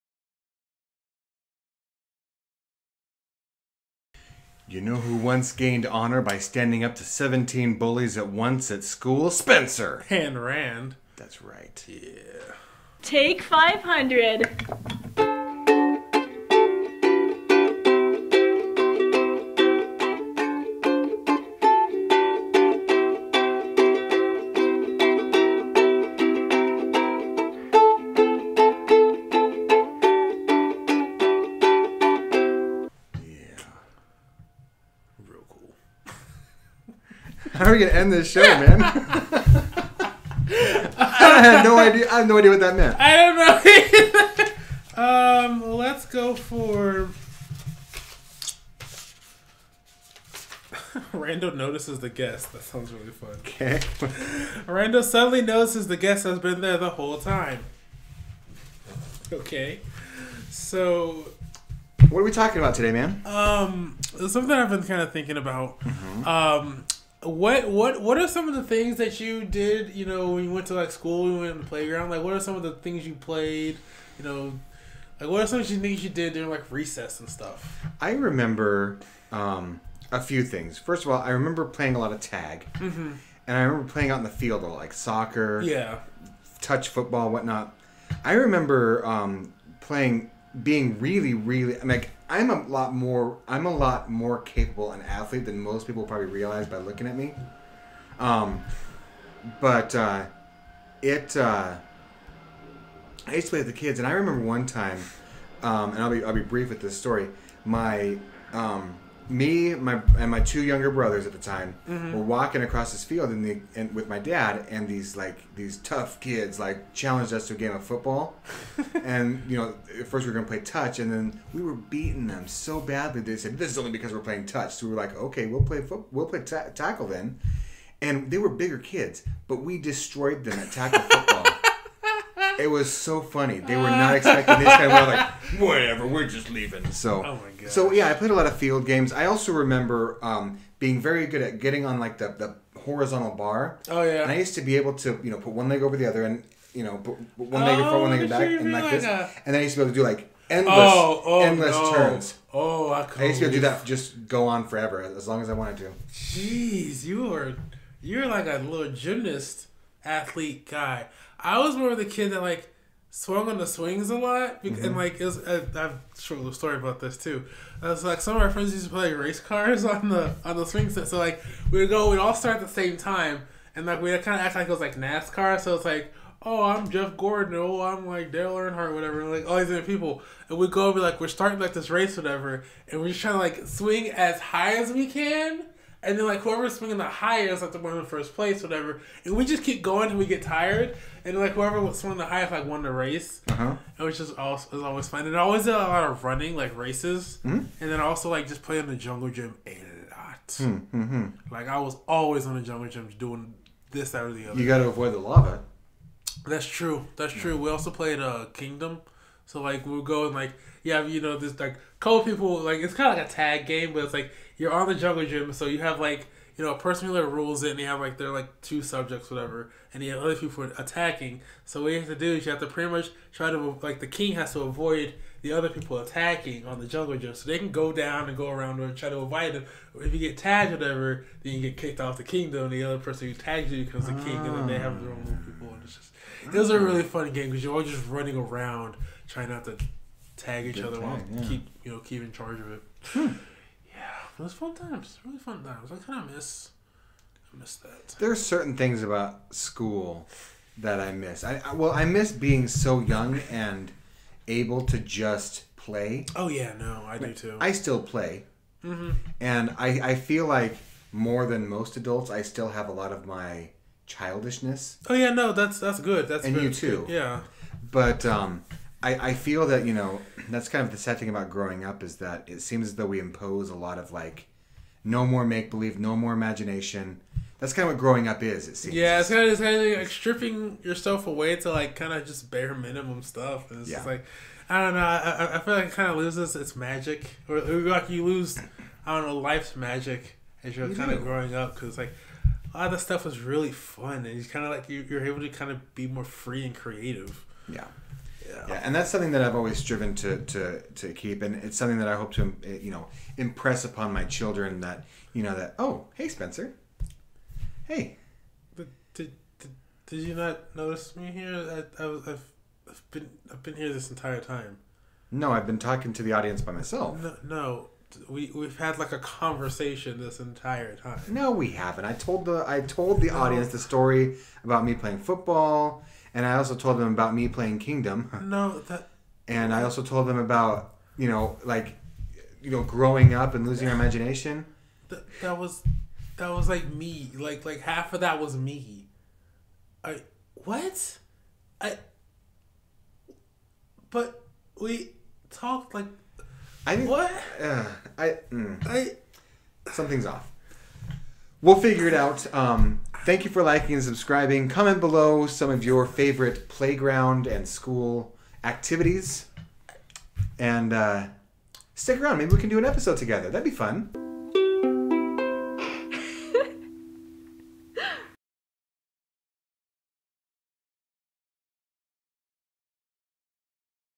Chatting about Playgrounds
Spencer and Rand chat about boyfriends and girlfriends! And Rand Episode 52!

Rampant Comedy brings you clean, silly comedy through this and other shows!

Featuring Spencer Lewis

Produced by Randall Bruner